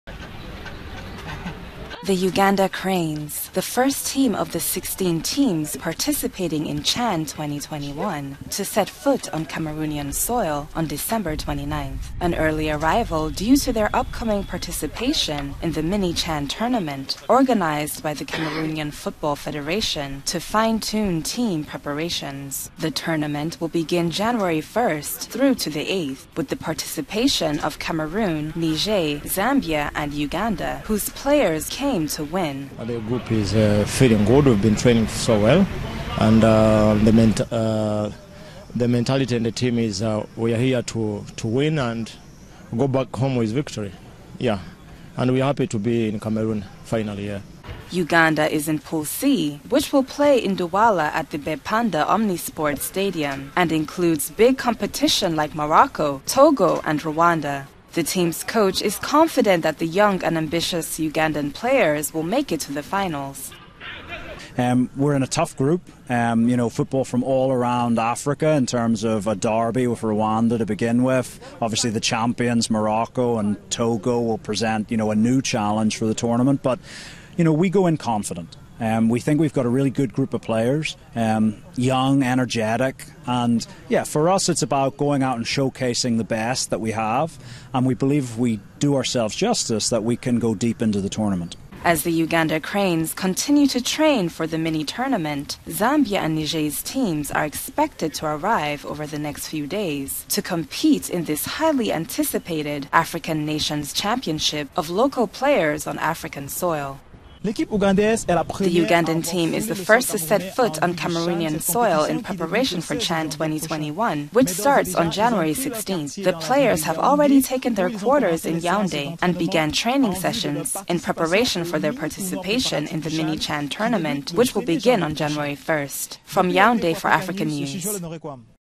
Uganda 🇺🇬 Cranes arrive in Cameroon ahead of CHAN2021
The Uganda Cranes, the first team of the 16 teams participating in CHAN 2020 to set foot on Cameroonian soil on December 29.